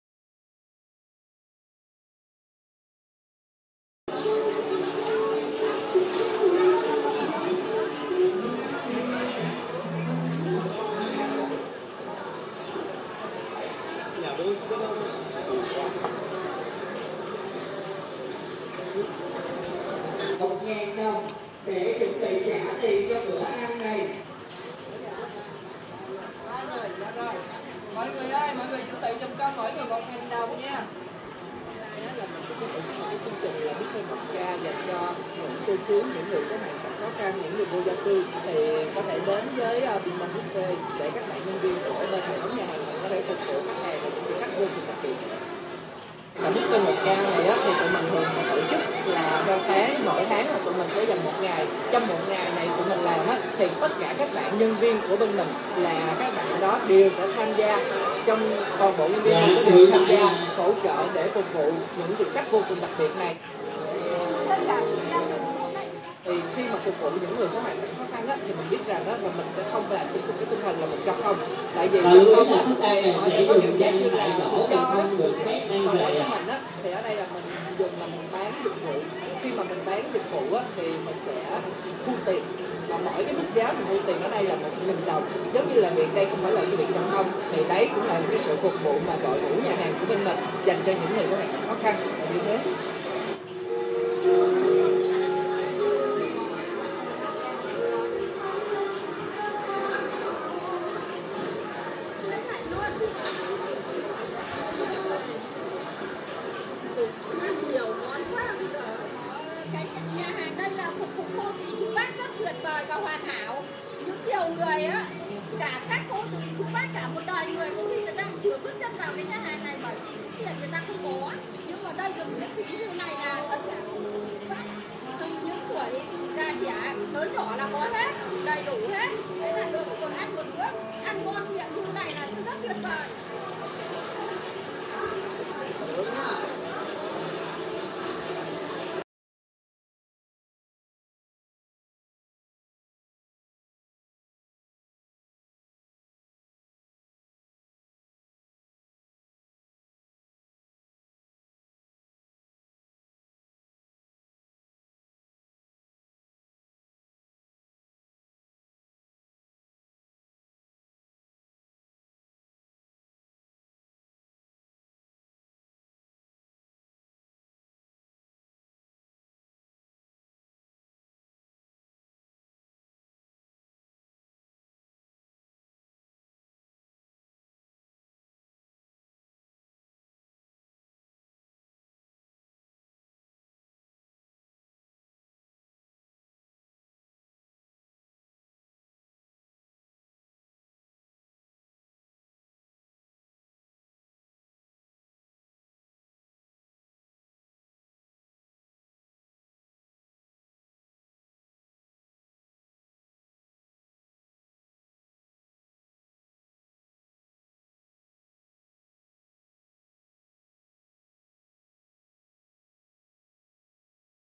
Xúc động tiệc buffet 1 000 đồng ở TPHCM
Chỉ với 1K (1.000 đồng), những người lao động nghèo, có hoàn cảnh khó khăn, người khuyết tật, người lớn tuổi… vẫn có thể thưởng thức bữa tiệc buffet với hàng chục món ăn đến no bụng thì thôi.